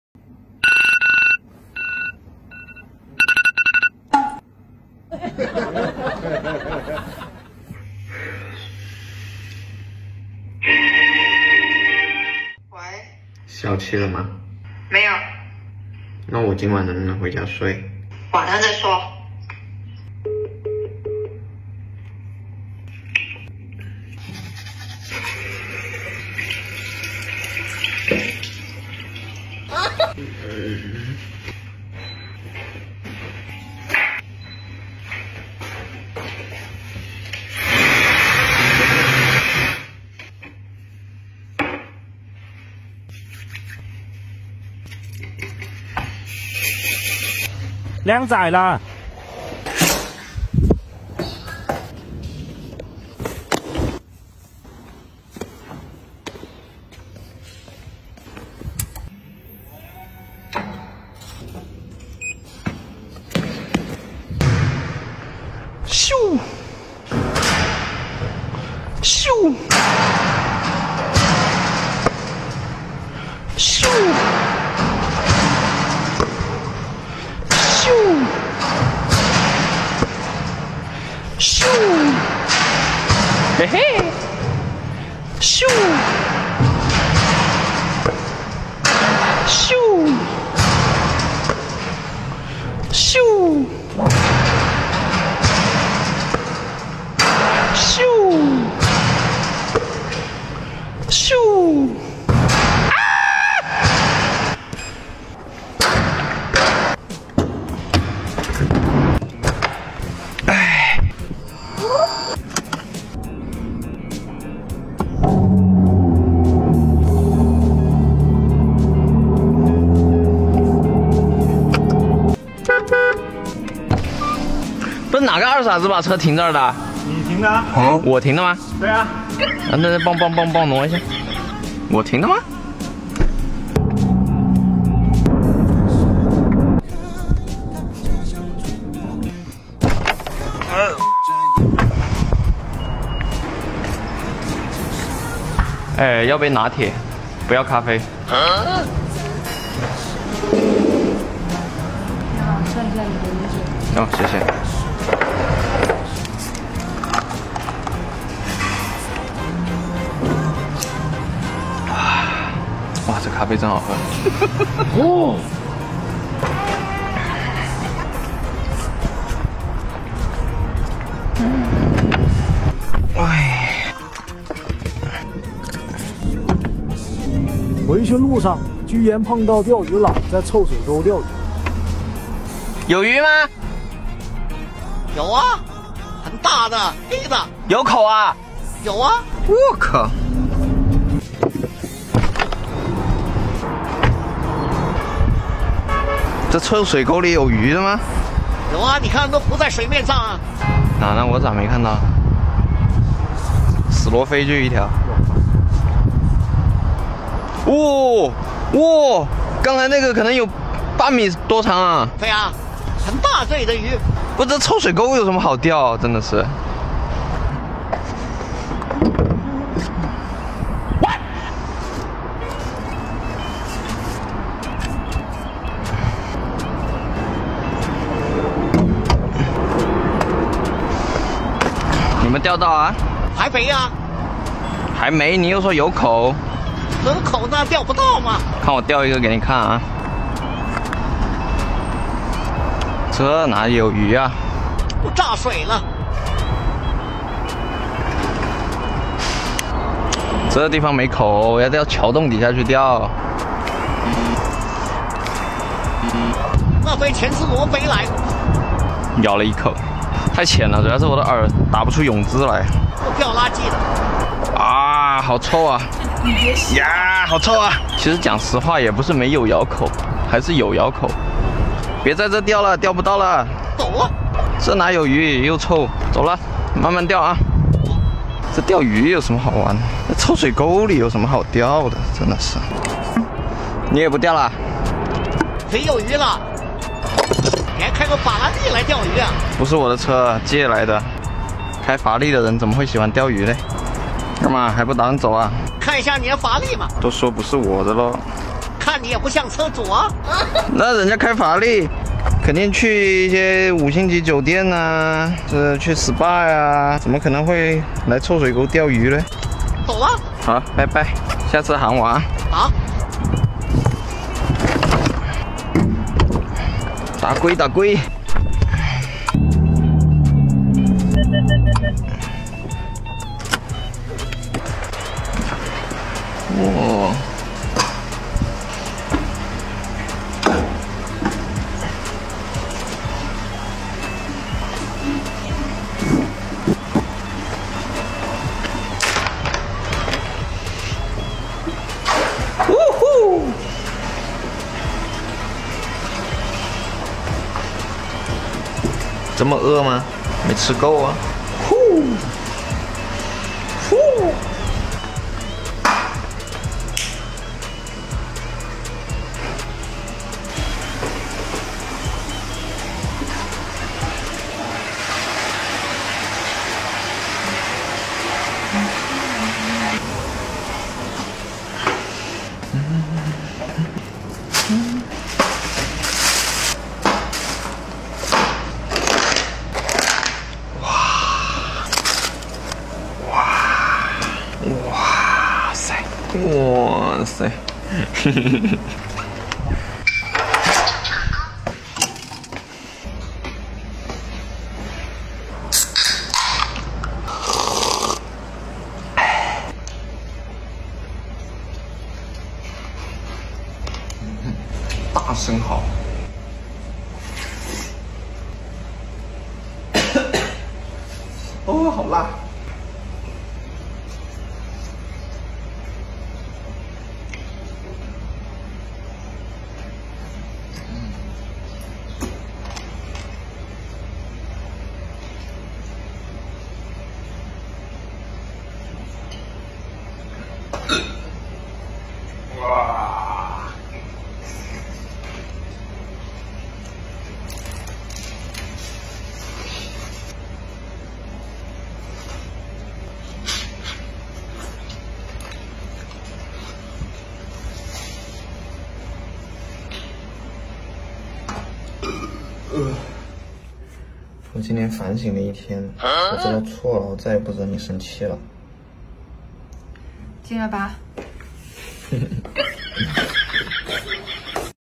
被老婆赶出家门的一天，充实且自在，就是不知道今天晚上还能回家吗！生活 搞笑 日常【小刚学长】
✦这里是小刚学长✦
一个玩车爱好者。
不解释，不回应，
做自己心中正确的事。